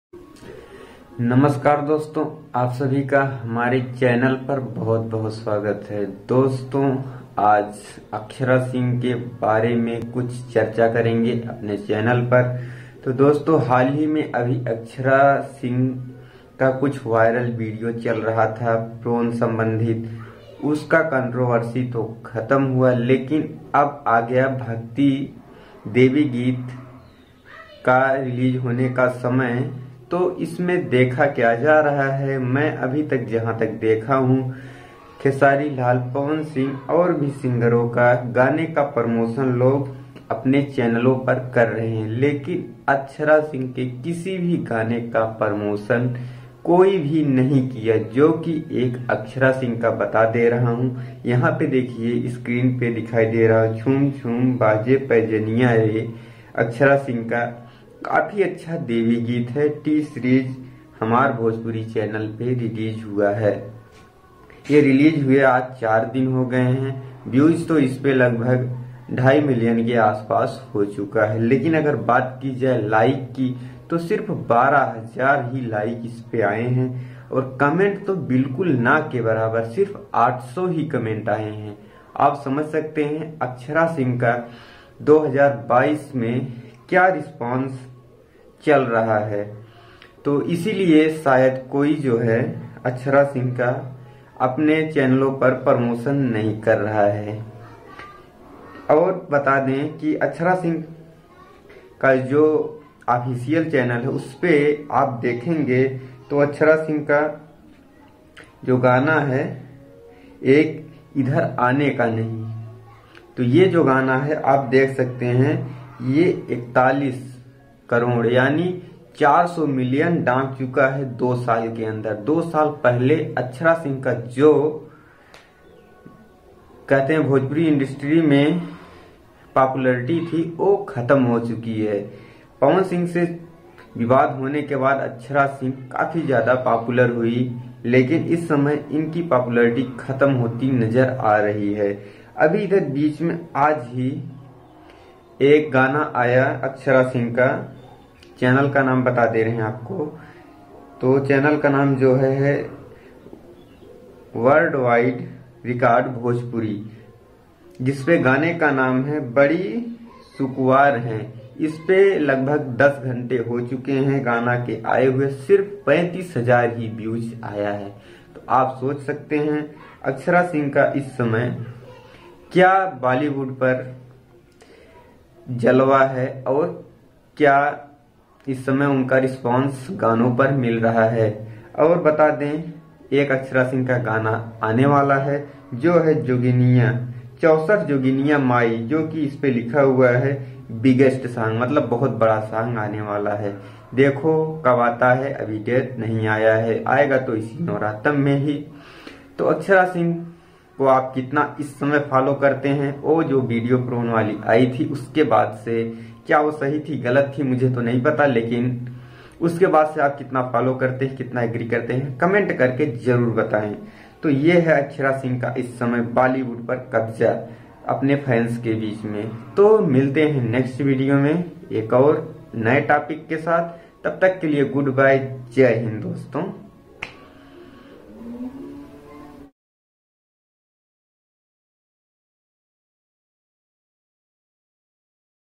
अक्षरा सिंह चल रही है फ्लॉप | जाने असली वजह | 2022 में एक भी गाने वायरल नहीं | Akshara Singh
अक्षरा सिंह चल रही है फ्लॉप जाने वजह | 2022 में एक भी गाने वायरल नहीं | Akshara Singh